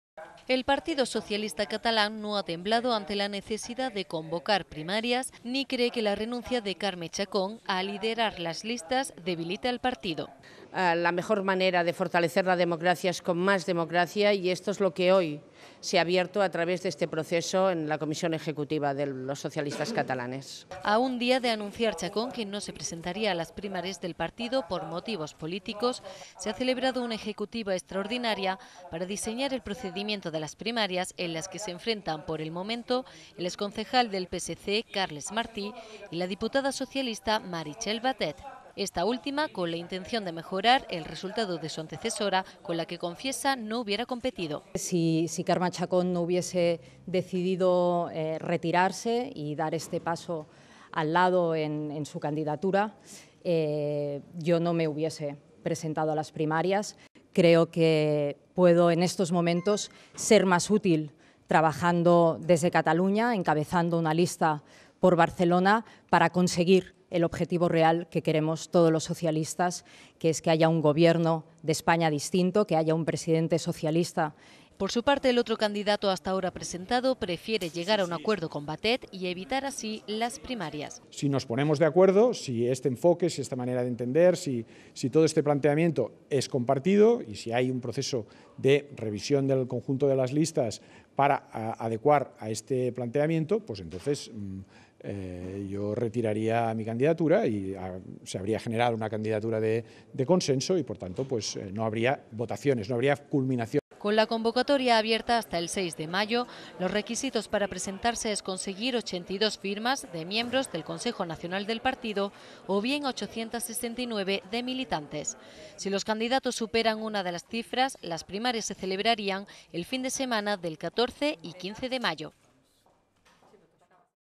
Batet y Martí como candidatos a las primarias de PSC
A un día de anunciar Carmé Chacón que no se presentaría a las primarias del partido por motivos políticos, se ha celebrado una ejecutiva extraordinaria para diseñar el procedimiento de las primarias, en las que se enfrentan, por el momento, el exconcejal  del PSC, Carles Martí y la diputada socialista, Maritxel Batet.